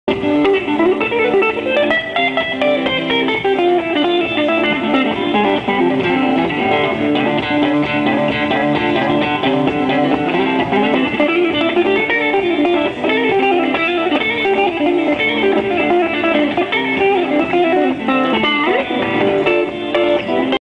Justin Johnson at Jessee's Coffee Bar in Carrboro, NC
Justin Johnson playing his guitar at Jessee's Coffee Bar in Carrboro, NC on April 10th, 2011 as part of the The Hint music festival!